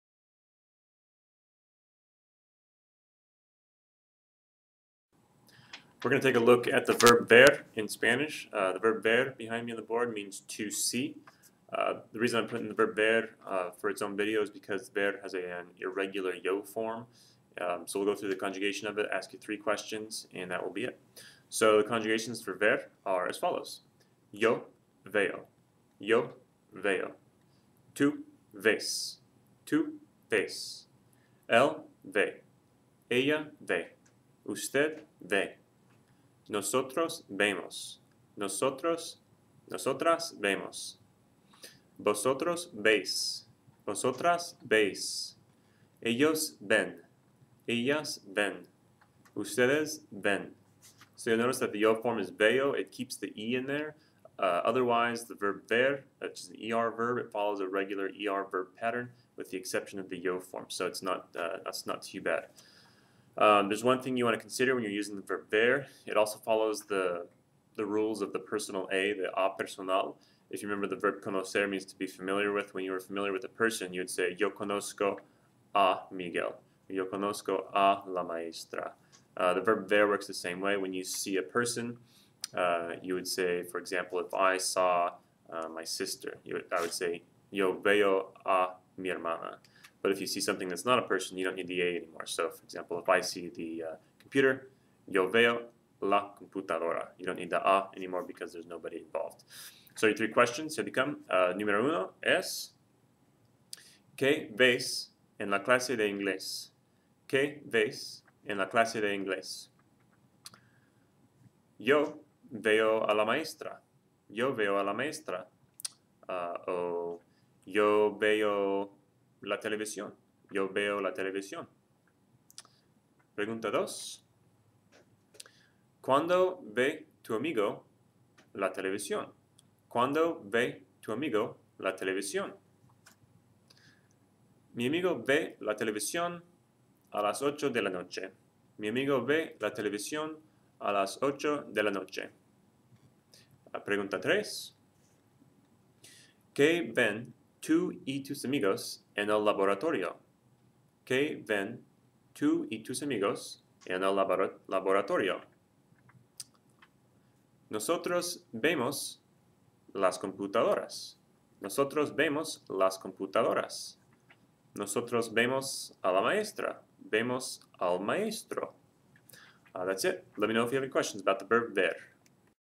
Spanish Lesson: Ver - To see
This video will explain how to conjugate the verb Ver - To See in the present tense in Spanish.  I will also ask 3 questions to give you a chance to practice.